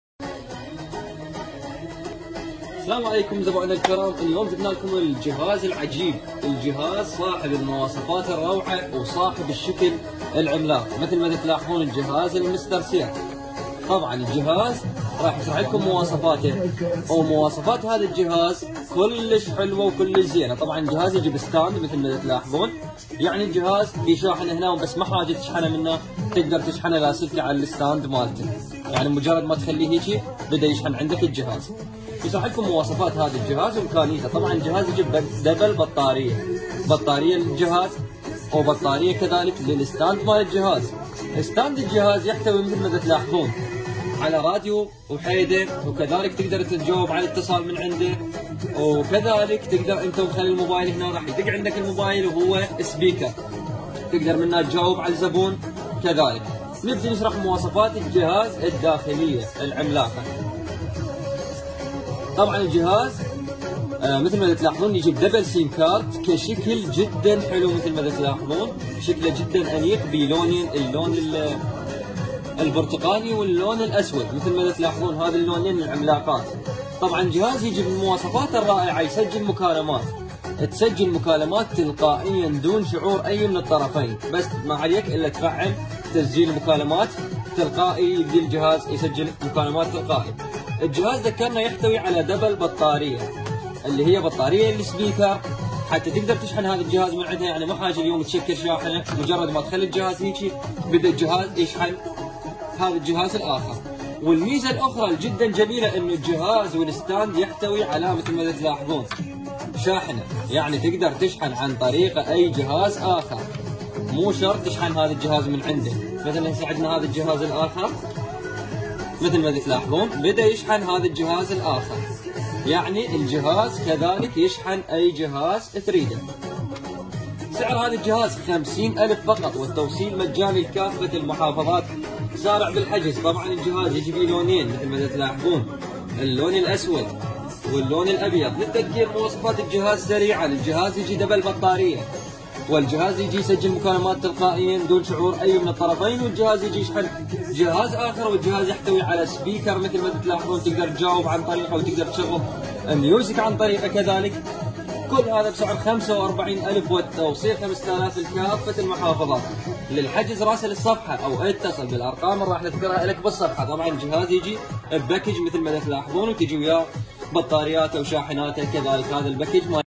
موبايل خدمي2 سيمكارت مع استاند بسعر تنافسي ب45 الف دينار فقط ⭕⭕
موبايل خدمي2 سيمكارت مع استاند
بسعر تنافسي45 الف دينار فقط  ⭕⭕⭕
والتوصيل 5
●تريد موبايل يأخذ 2 خطوط ورام😍💪
●صوت عالي 3دي
●تريد مبايل يشحن مبايلك الثاني
اتريد موبايل يشحن لاسلكي
●تريد جهاز بي تسجيل مكالمات تلقائي
●تريدموبايل يحتوي على دبل بطارية كبيره توفرلك الشحن لفترات طويله
●يحتوي على بلوثوث وراديو وكاميرا
للمزيد من التفاصيل والمميزات مشاهدة الفيديو 📽
يوجد توصيل لكافة محافظات العراق 🚗🚗